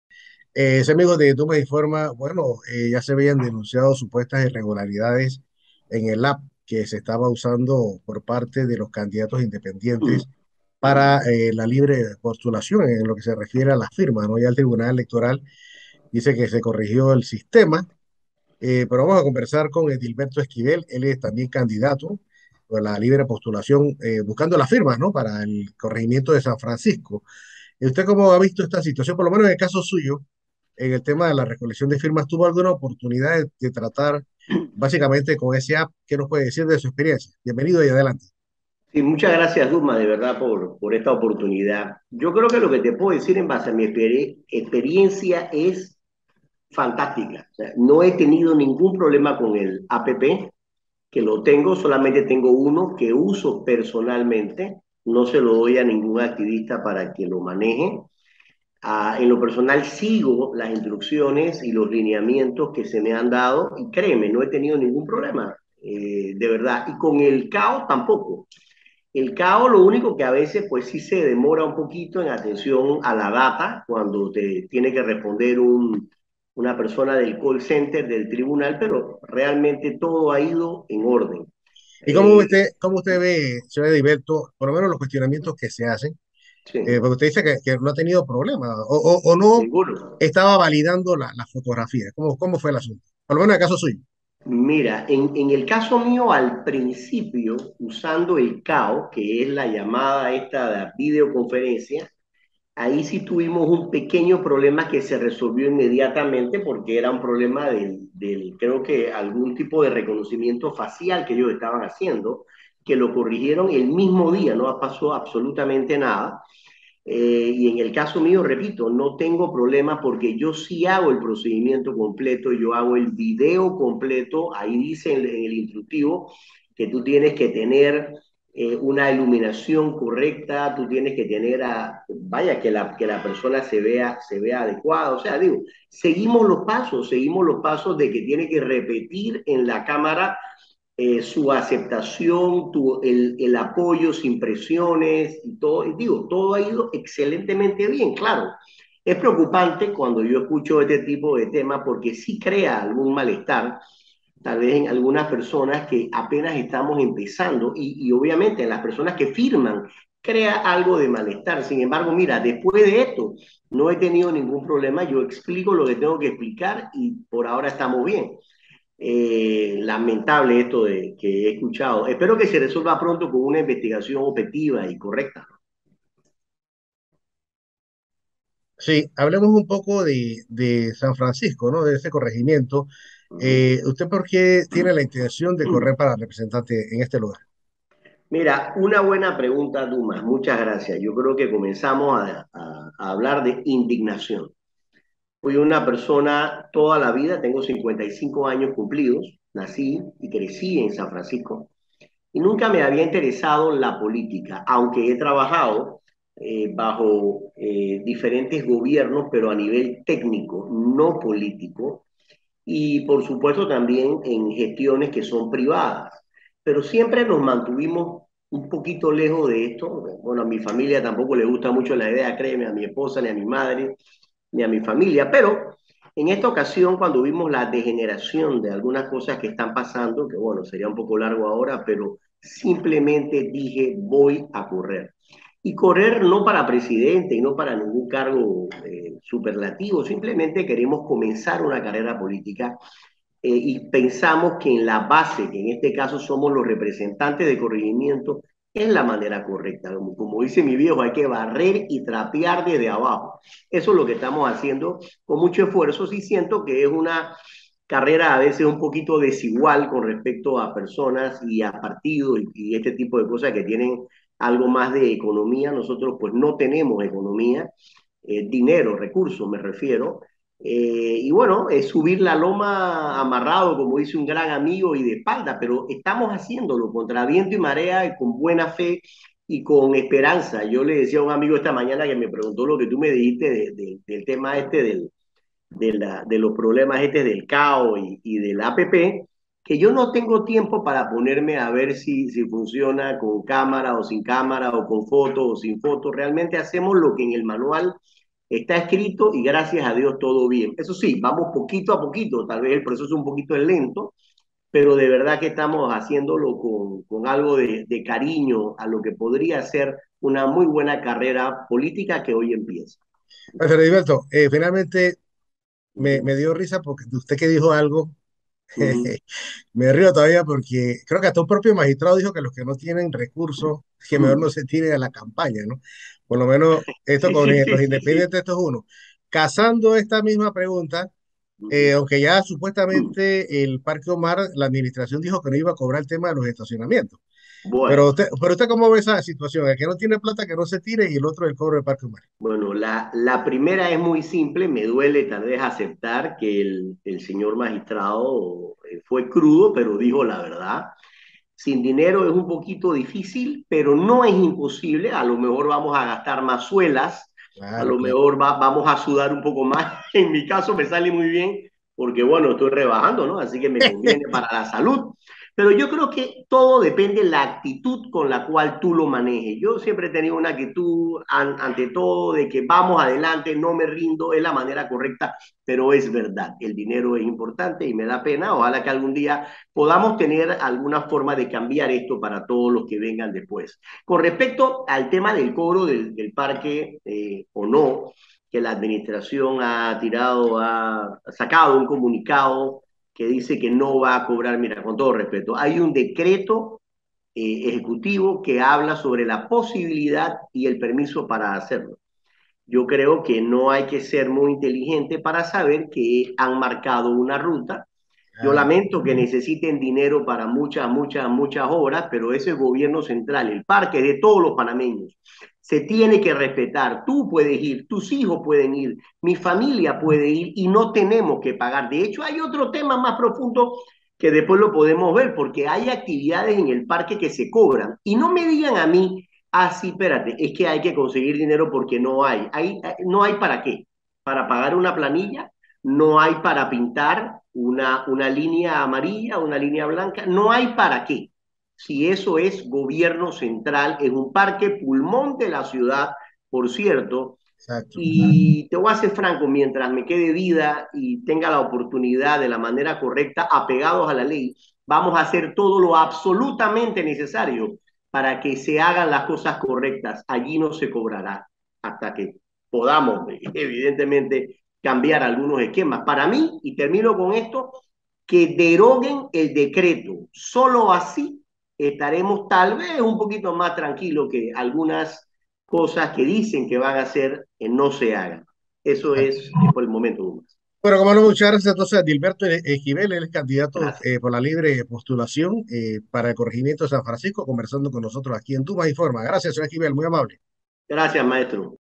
San Francisco está cansado del letargo en que se encuentra donde no hay beneficio para sus moradores
Al parecer no todos los candidatos independientes por la libre postulación que se encuentran en el proceso de recolección de firmas han tenido dificultades con la aplicación de dispositivos móviles que usan sus activistas y que será motivo de una denuncia por parte del Tribunal Electoral a la Fiscalía electoral.
El precandidato independiente por el Corregimiento de San Francisco en la ciudad capital, Edilberto Esquivel dijo que tanto él como sus activistas no han tenido ningún inconveniente y a la vez dijo que decidió buscar firmas porque la comunidad esta cansada del letargo en que se encuentra dicha junta y es hora de hacer los cambios correspondientes en beneficio de sus moradores.